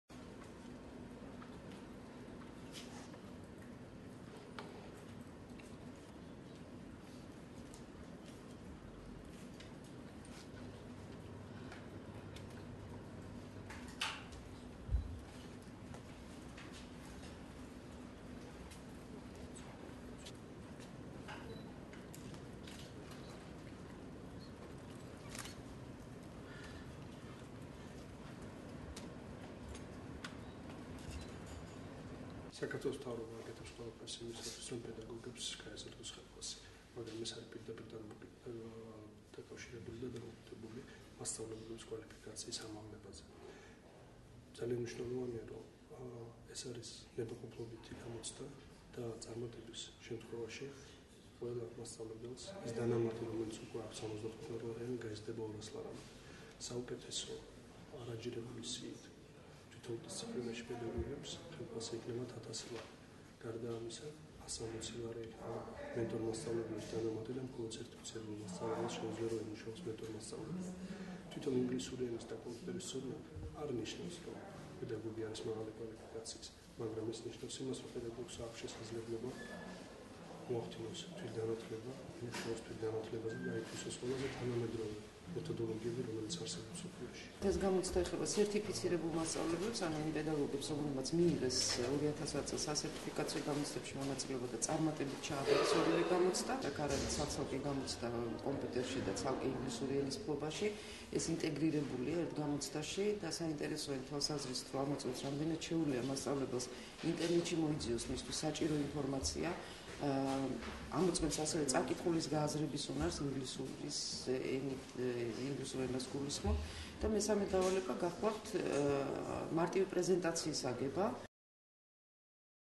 inglisurisa da kompiuteris gamocda sertificirebul pedagogebisTvis
saqarTvelos ganaTlebisa da mecnierebis ministri dimitri SaSkini da gamocdebis erovnuli centris direqtori maia miminoSvili  inglisurisa da kompiuteris integrirebuli gamocdis mimdinareobas gaecnen.  inglisuri enisa da kompiuteris gamocdaSi mxolod sertificirebuli  maswavleblebi monawileoben.

sagamocdo testis davalebebi isea agebuli, rom erTdroulad mowmdeba rogorc inglisuris, ise kompiuteris codna. kandidati gamocdas kompiuteris saSualebiT Caabarebs.

aRsaniSnavia, rom inglisuri enisa da kompiuteris gamocdaSi monawile  kandidats moeTxoveba, inglisur enaze kiTxvis unars saSualoze dabal doneze (pre-intermediate level) flobdes. inglisuri enis codnis es done Seesabameba evrosabWos mier dadgenili ucxouri enebis codnis A2 + dones da kembrijis universitetis KET (Key English Test) gamocdis dones.

inglisurisa da kompiuteris integrirebuli gamocda wels pirvelad tardeba da misi Cabarebis survili 717 -ma sertificirebulma pedagogma gamoTqva.